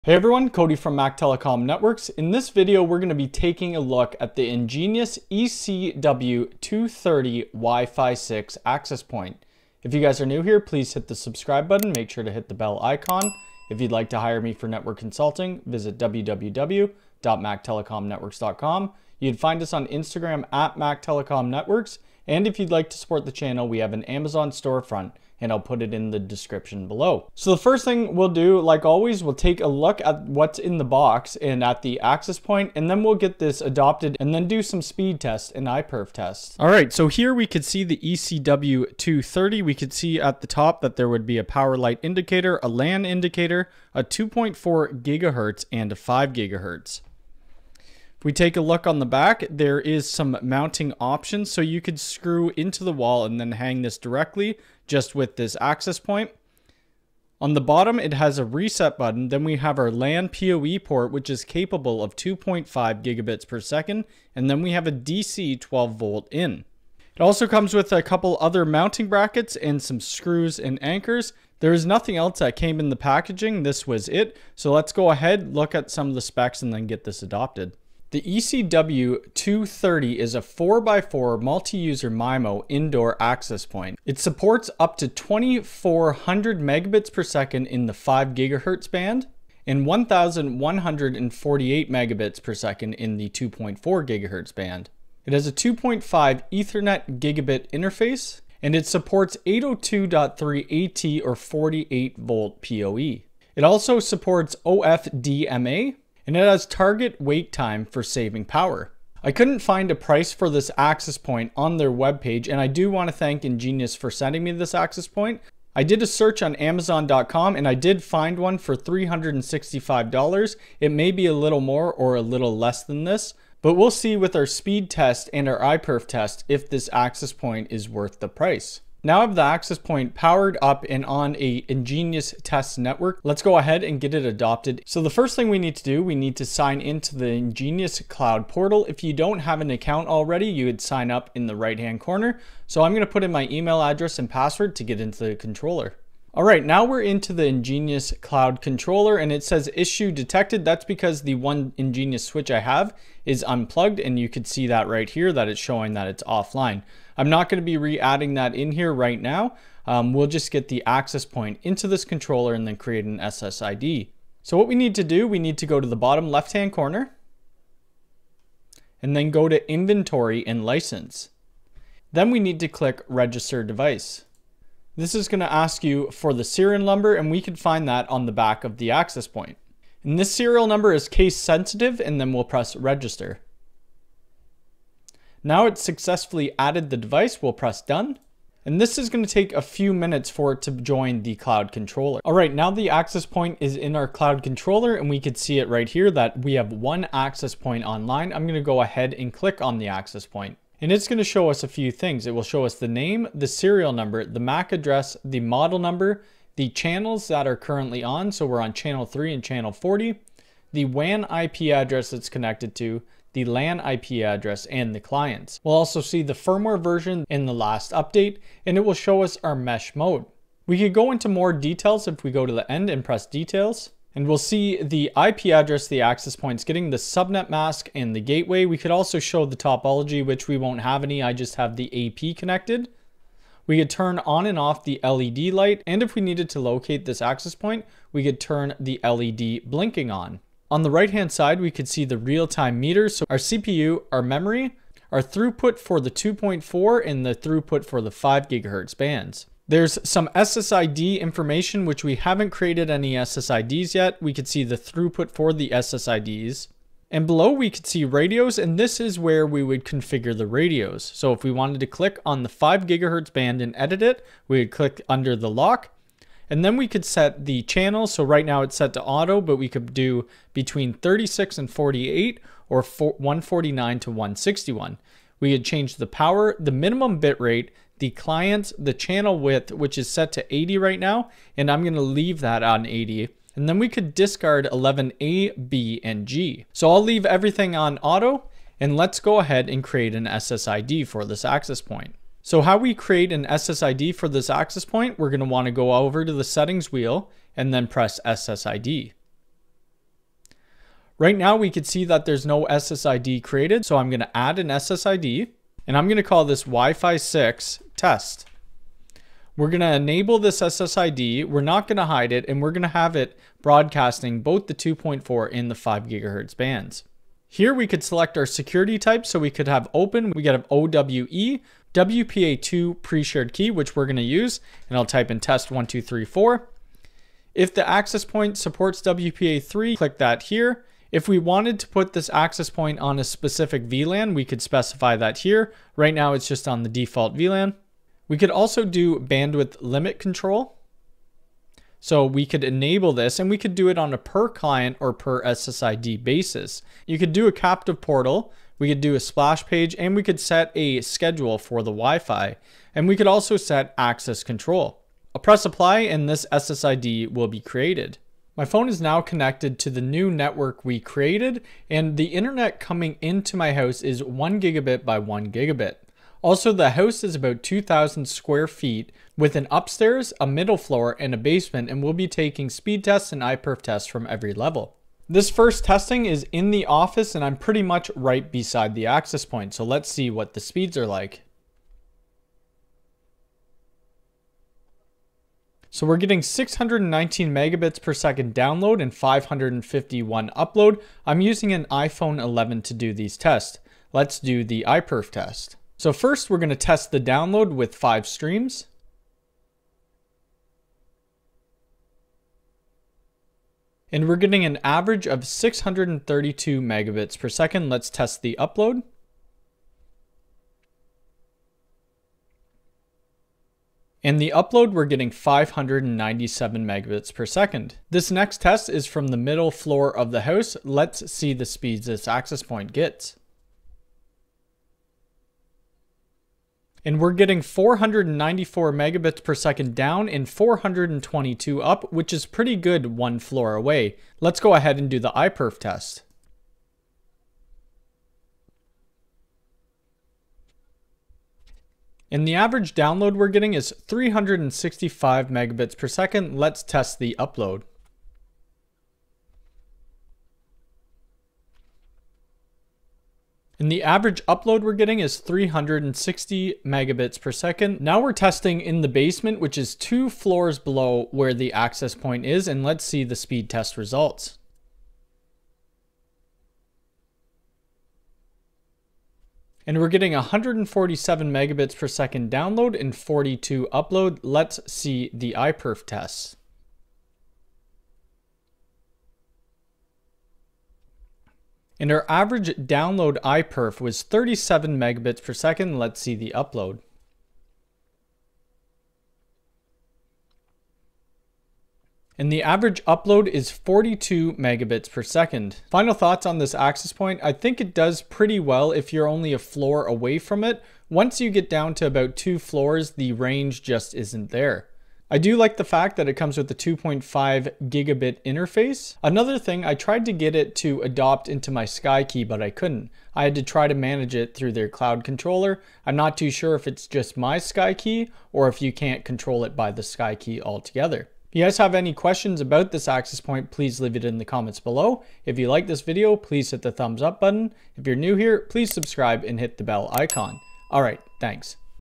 EnGenius ECW230 WIFI 6 AP
In this video we take a look at the Engenius ECW230 WIFI 6 AP. This access point features 802.11ax WIFI 6 and has a 2.5gb ethernet interface.

◾Canadian Amazon Store front:

◾USA Amazon store front:

◼️Timestamps